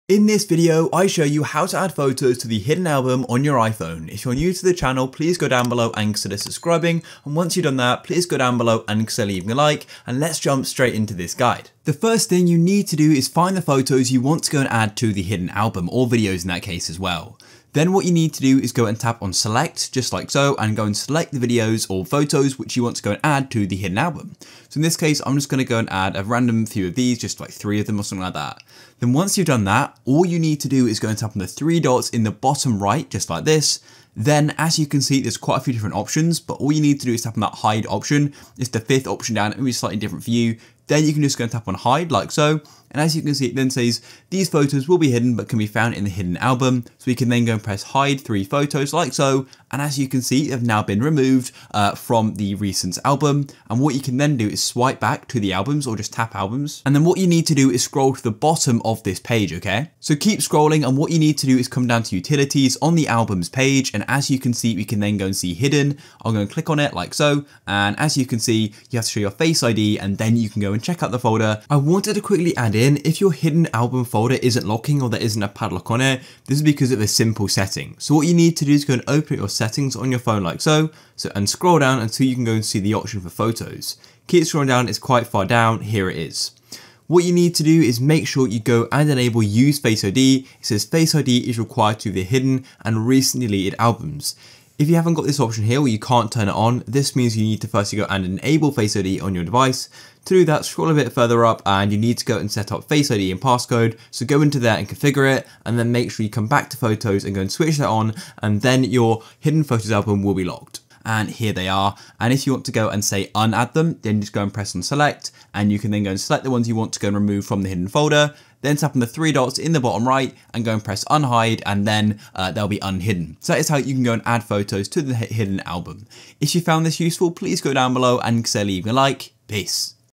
How To Add Photos To Hidden Album On iPhone - Full Guide
I show you how to add photos to hidden album on iphone and how to add photos in hidden album in iphone in this video.

GuideRealm